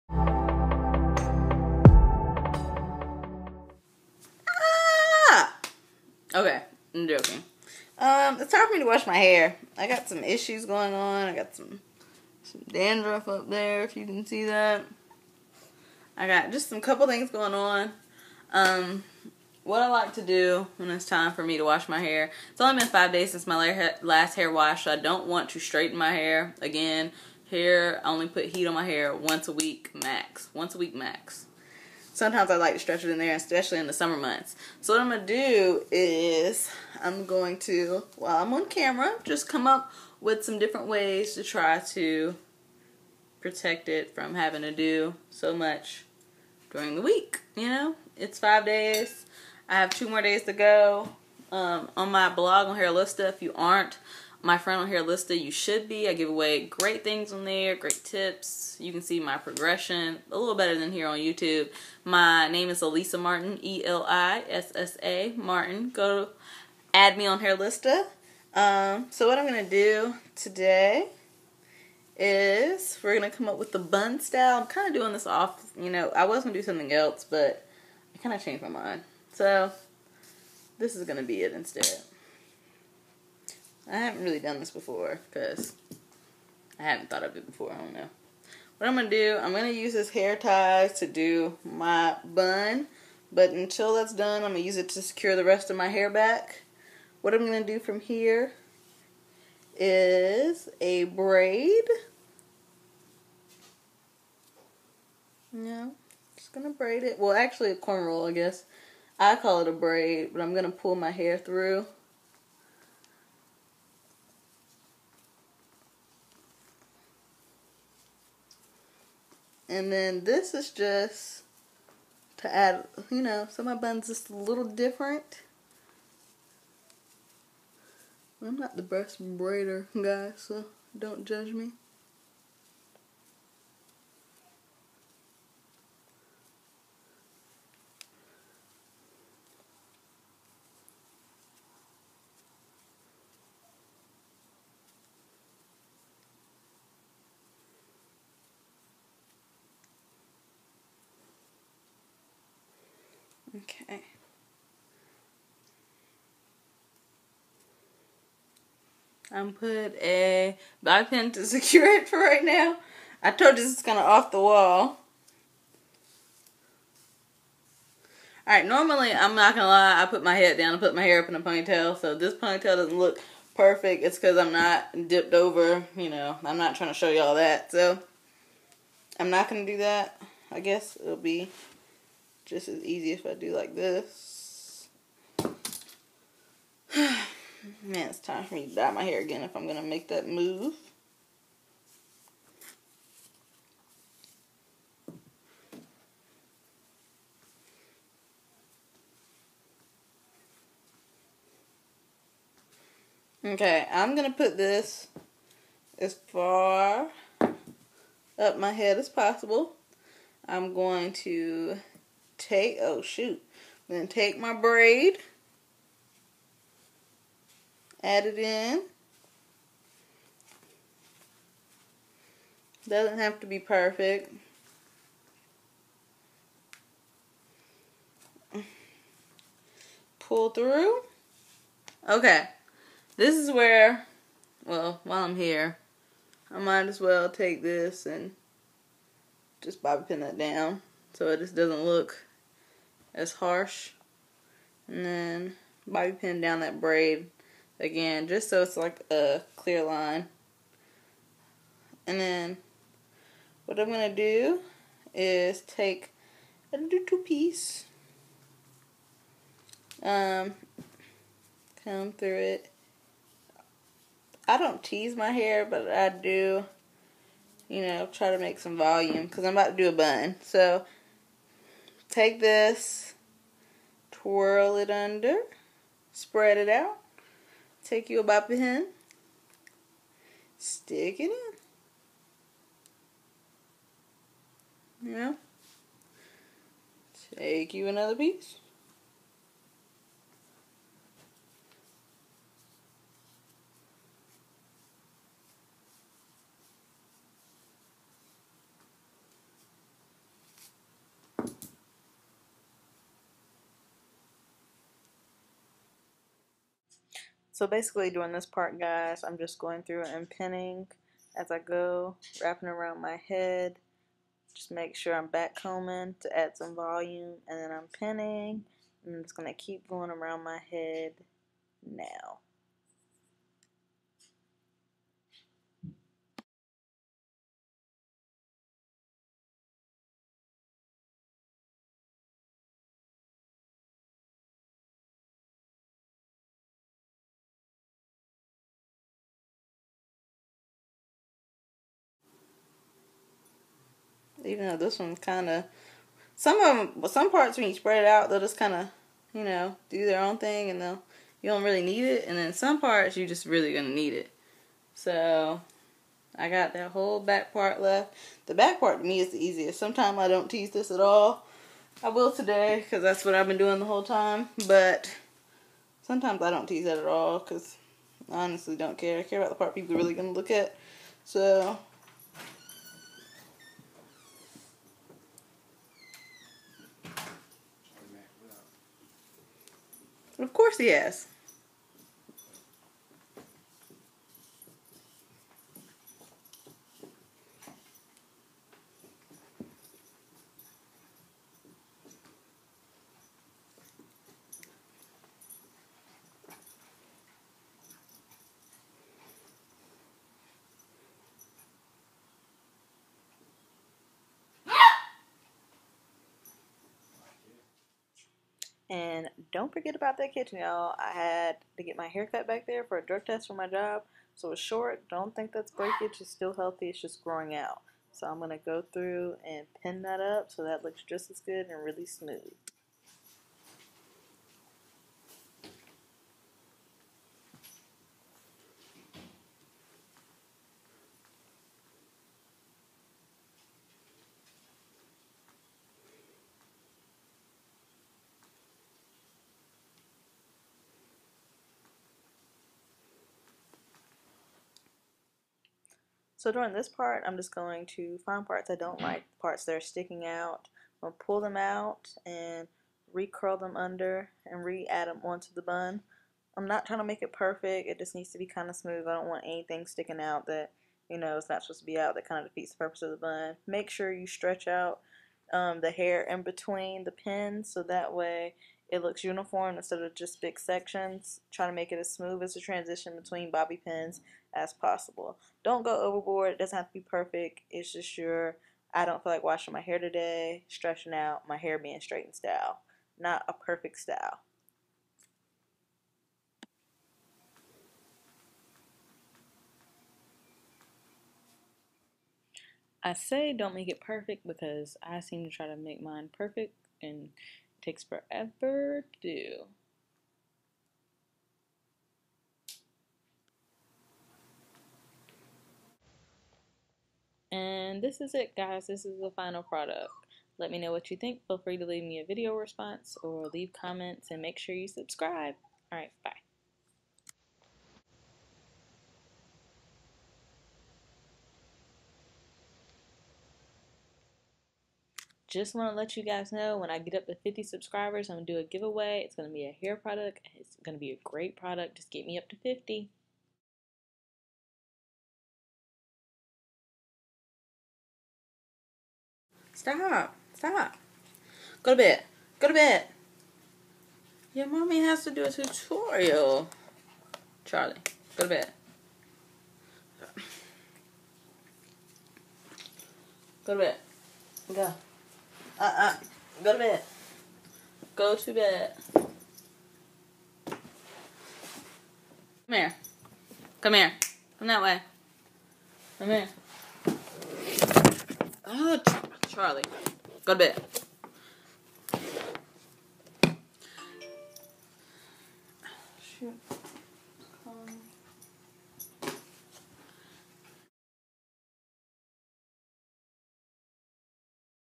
Tutorial: Braided Bun
Hey Everyone,

This is my tutorial on a "messy" hair bun with a braid going into the bun. This was kind of off the off the cuff! I hope you enjoy!

Also:

Add Me

Hairlista.com/profile/elissamartin